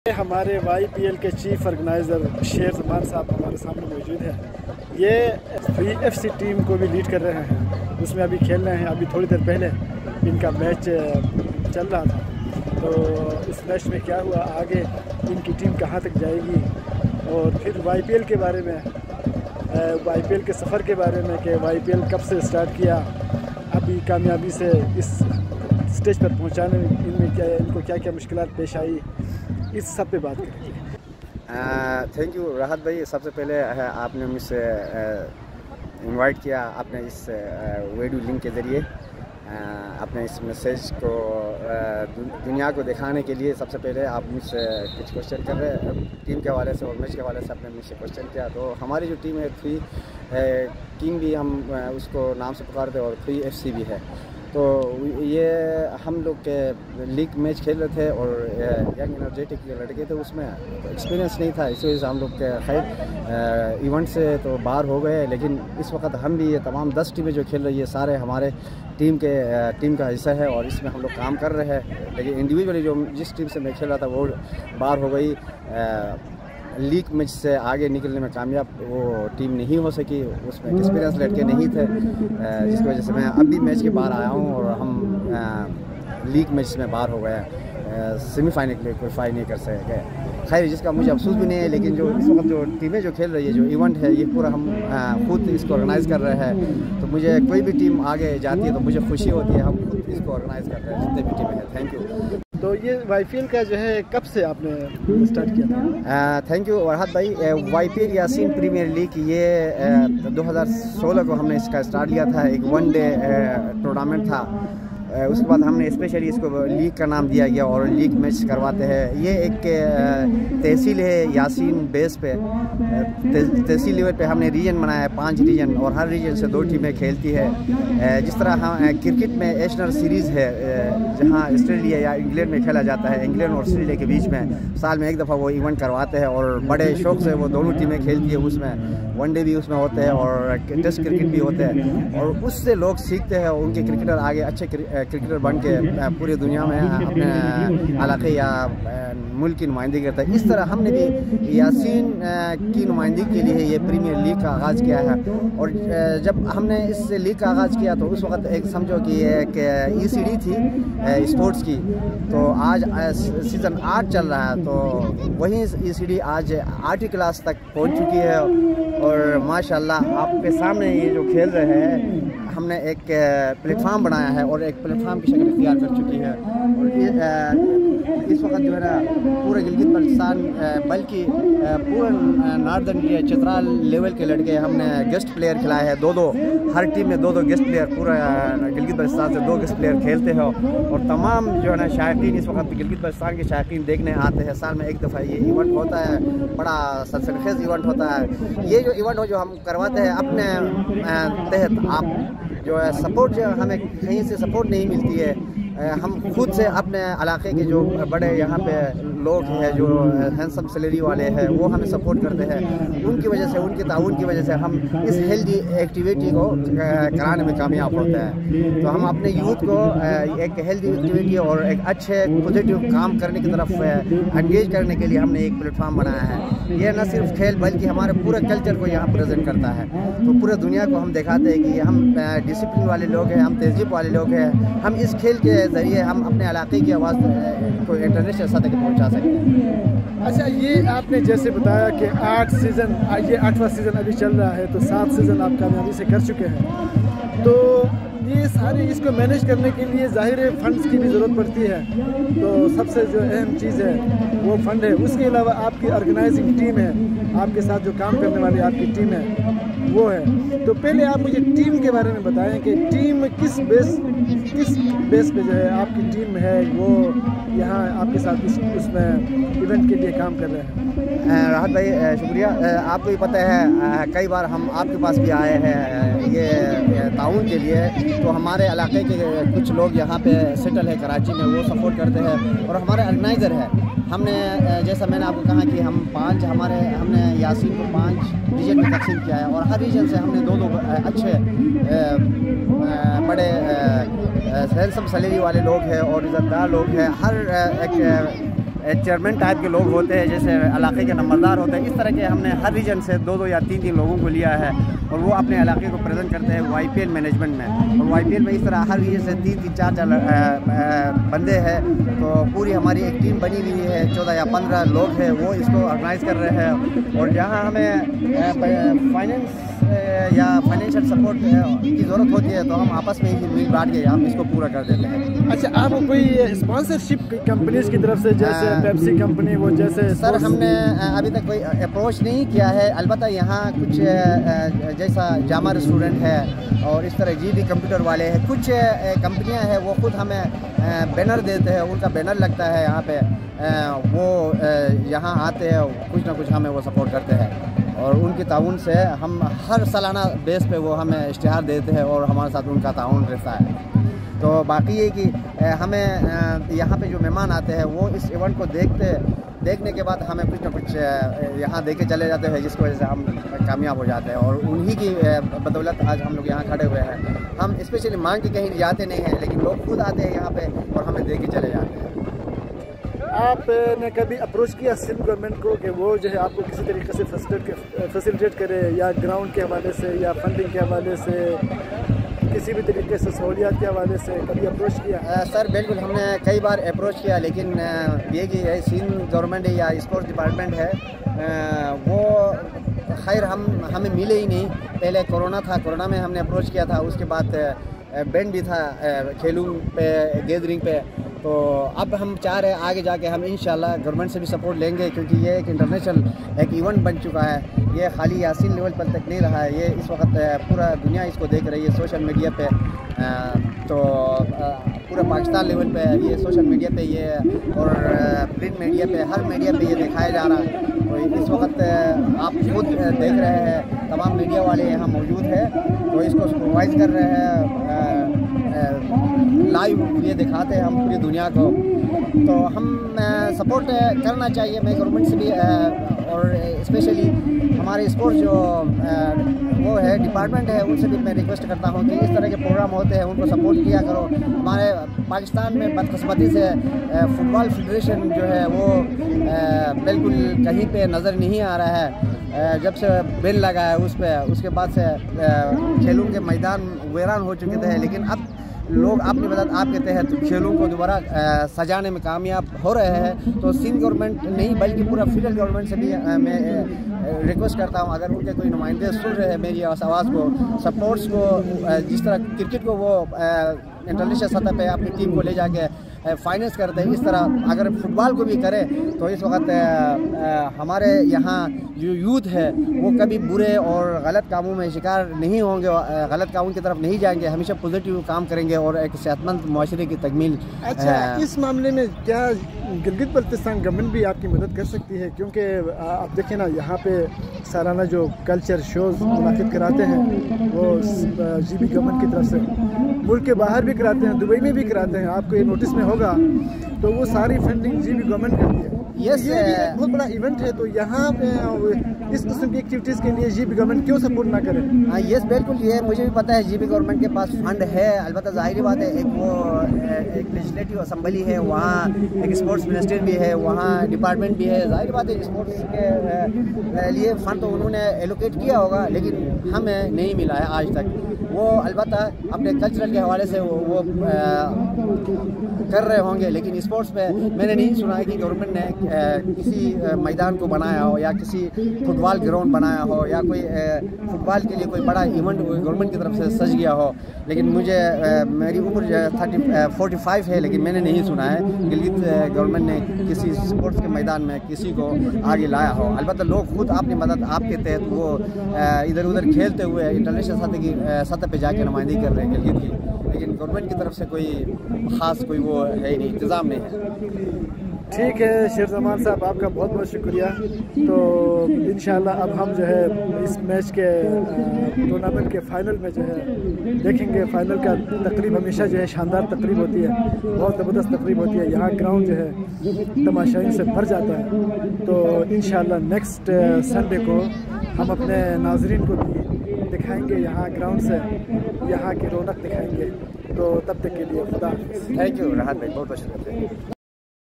Interview | Sher Zama, organiser YPL
کراچی میں گلگت بلتستان کے طلبہ کا اہم فٹ بال ٹورنامنٹ یاسین پریمیئر لیگ کے سرپرست اعلیٰ شیر زمان کا خصوصی انٹرویو